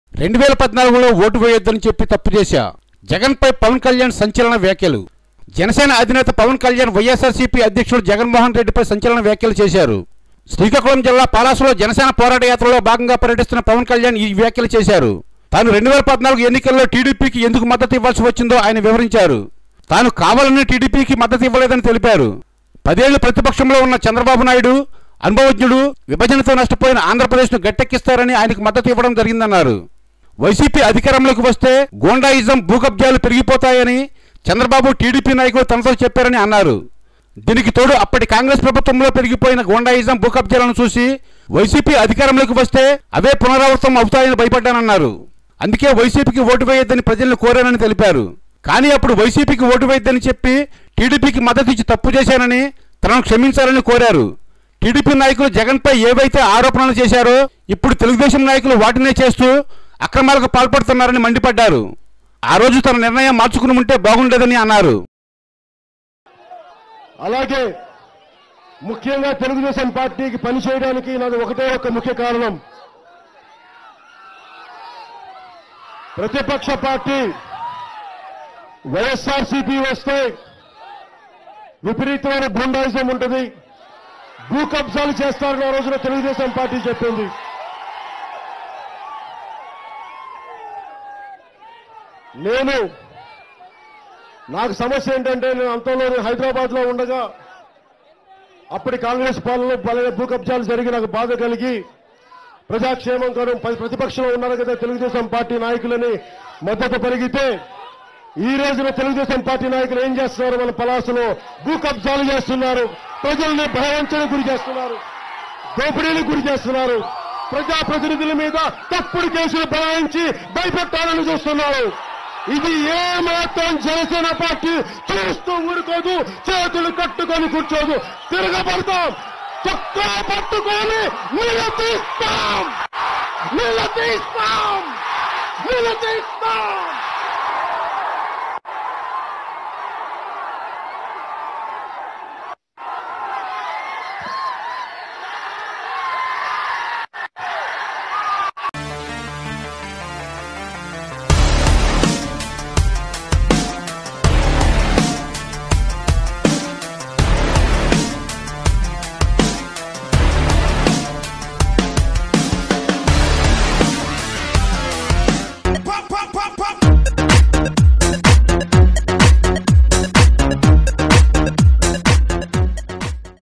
వైఎస్ జగన్ పై పవన్ కళ్యాణ్ సంచలన వ్యాఖ్యలు| Pawan Kalyan shocking Comments on YS Jagan| Pawan Speech
Janasena Chief Pawan Kalyan has made sensational comments on YSR Congress Chief YS Jagan over TDP Alliance in Previous Elections.
Watch: Pawan Kalyan Speech
Amaravathi Media